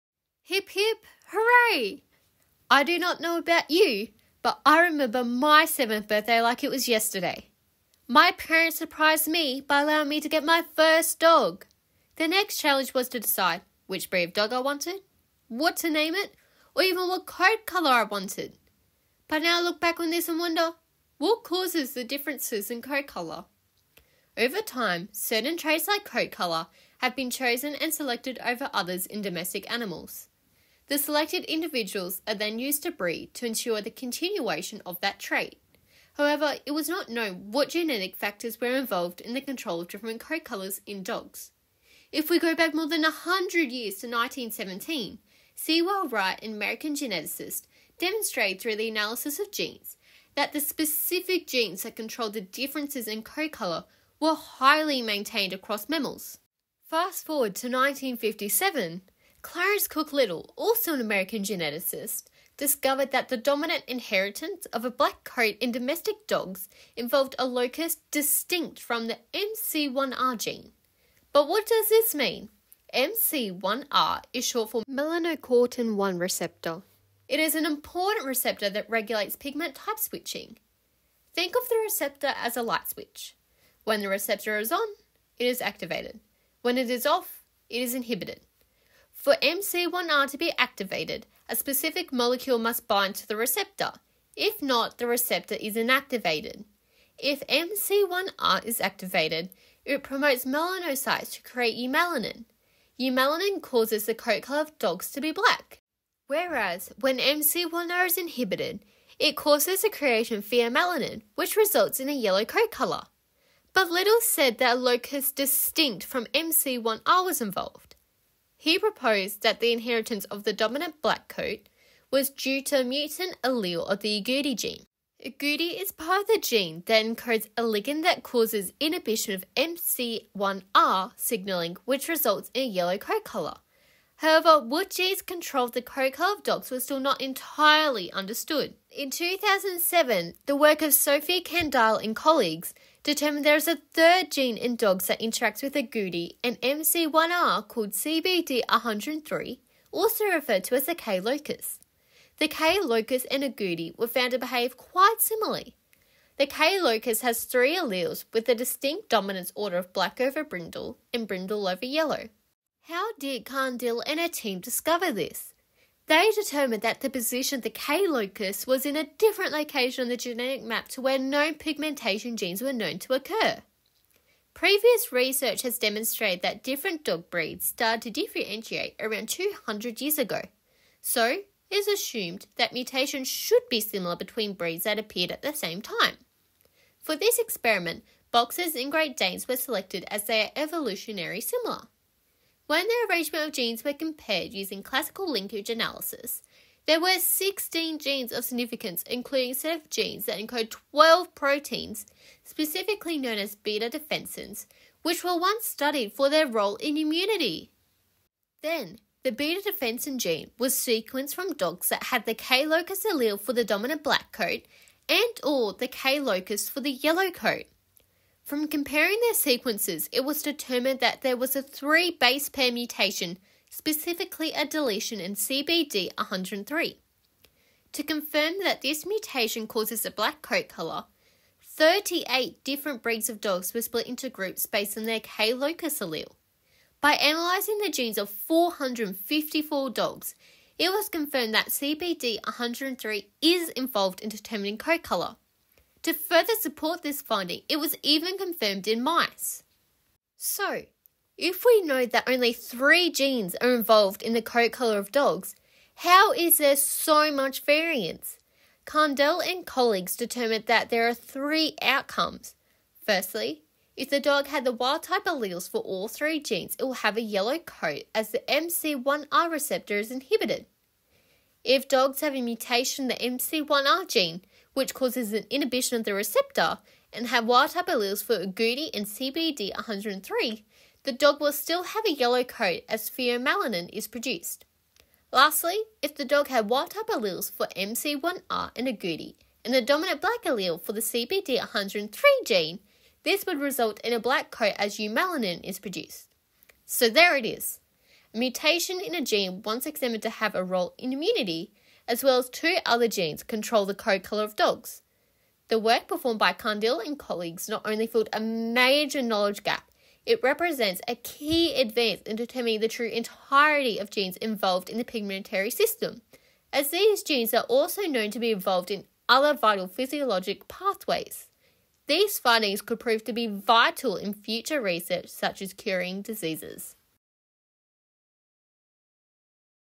Immunity genes also control a dog’s coat colour?
A β-defensin mutation causes black coat color in domestic dogs. Candille SI, Kaelin CB, Cattanach BM, Yu B, Thompson DA, Nix MA, Kerns JA, Schmutz SM, Millhauser GL, Barsh GS. Science. 2007 Nov 30;318(5855):1418-23. doi: 10.1126/science.1147880.

In 1917 Sewall Wright demonstrated through the analysis of genes that the specific gene that controlled the differences in coat colour was highly maintained across mammals. Forty years later Clarence Cook Little determined that two genes were involved in the control of different coat colours in dogs. The first gene is called Agouti and encodes a ligand that causes the inhibition of the product of second gene that encodes a receptor called the Melanocortin 1 receptor (Mc1r). When Mc1r is inhibited, it causes the creation of pheomelanin, which results in a yellow coat colour. When Mc1r is activated, it causes the production of melanocytes which synthesise eumelanin, resulting in a black coat colour.

The work of Sophie Candille and colleagues in 2007 extended on the previous research by Sewall Wright and Clarence Cook Little, revealing that there are three main genes involved in determining a dog’s coat colour. Through the analysis of genes from 454 individual dogs selected from between 38 breeds, it was determined that a mutation in a gene once investigated for its role in immunity was the cause of the variance in dogs’ coat colour. This mutation was a three-base pair deletion in the CBD103 gene. Depending on what type of alleles a dog has for Agouti, Mc1r, and CBD103 genes determines its coat colour. Candille and colleagues determined there are three possible outcomes. If the dog had the wild-type alleles for all three genes it will have a yellow coat as the Mc1r receptor is inhibited causing pheomelanin to be produced. If dogs have a mutation in the Mc1r gene and had wild-type alleles for Agouti and CBD103, the dog will have a yellow coat as pheomelanin is produced. Lastly, if the dog had wild-type alleles for Mc1r and Agouti and the dominant black allele for the CBD103 gene, this would result in a black coat as eumelanin is produced. These findings not only solved a major genetic mystery but could also prove to be vital in future research for diseases, human skin pigmentation, and immunology to name a few.

Creator: Laura Steinhardt

References:
Candille SI, Kaelin CB, Cattanach BM, Yu B, Thompson DA, Nix MA, Kerns JA, Schmutz SM, Millhauser GL, Barsh GS. A β-defensin mutation causes black coat color in domestic dogs. Science. 2007;318(5855):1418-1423.
Geng LN, Yao Z, Snider L, Fong AP, Cech JN, Young JM, van der Maarel SM, Ruzzo WL, Gentleman RC, Tawil R, Tapscott SJ. DUX4 activates germline genes, retroelements, and immune mediators: implications for facioscapulohumeral dystrophy. Dev Cell. 2012;22(1):38-51.
Sampson J, Binns MM. In The Dog and its Genome. Edited by Ostrander EA, Giger U, Lindblad-Toh K. Cold Spring Harbor, N.Y. : Cold Spring Harbor Laboratory Press. 2005:19-30.
Semple F, Dorin JR. β-Defensins: multifunctional modulators of infection, inflammation and more? J Innate Immun. 2012;4(4):337-348.
Semple F, Webb S, Li HN, Patel HB, Perretti M, Jackson IJ, Gray M, Davidson DJ, Dorin JR. Human beta-defensin 3 has immunosuppressive activity in vitro and in vivo. Eur J Immunol. 2010;40(4):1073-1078.
Sturm RA. Molecular genetics of human pigmentation diversity. Hum Mol Genet. 2009;18(R1):R9-17.
Wright S. Color inheritance in mammals: VII, The horse – Studbooks have afforded data for many investigators – Many different pairs of allelomorphs to be found in all combinations – Some interesting cases in hybrids. J Hered. 1917;8(12):561-564.